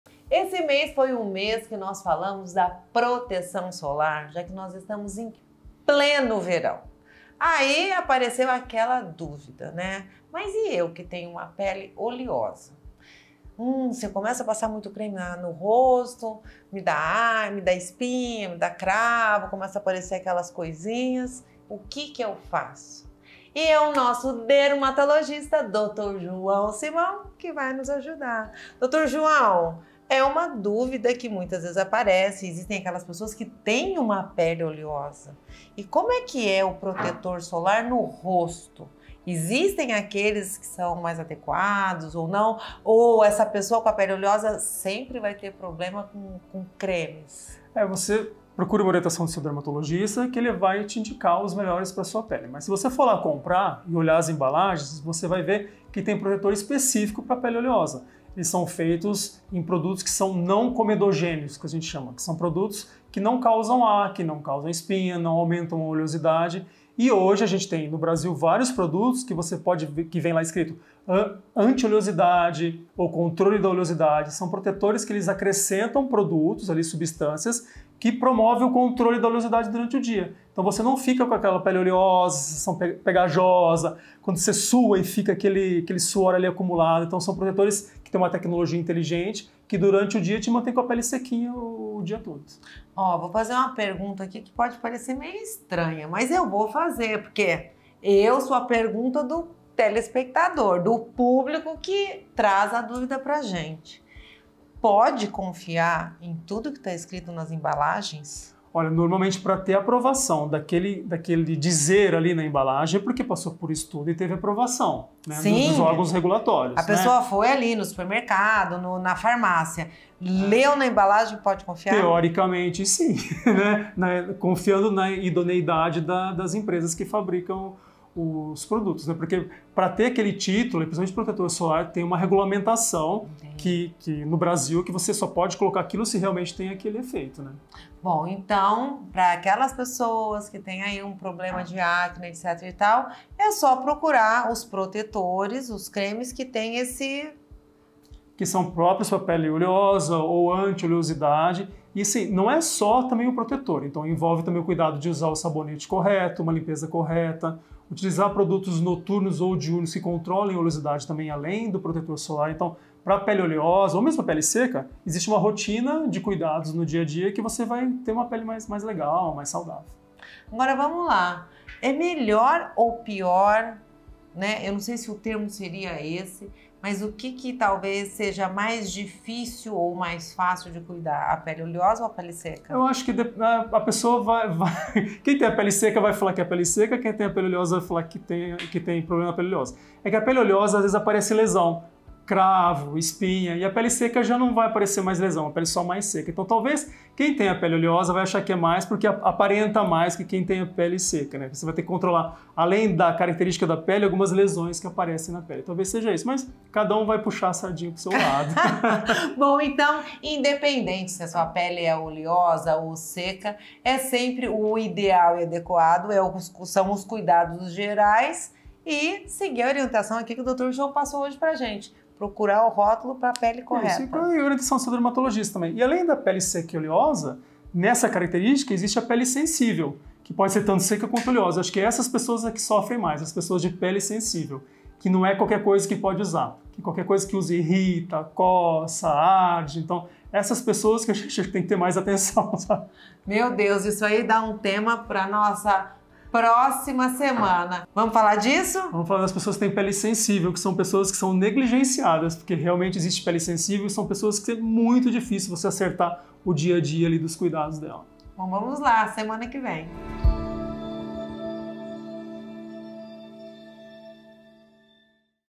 Protetor solar ideal para pele oleosa: como escolher e aplicar
A pele oleosa é um tipo de pele que produz mais sebo do que o normal. Isso pode causar o aparecimento de brilho, poros dilatados e espinhas.

O uso de protetor solar é essencial para todas as pessoas, independentemente do tipo de pele. No entanto, as pessoas com pele oleosa precisam escolher um protetor solar que seja adequado para o seu tipo de pele.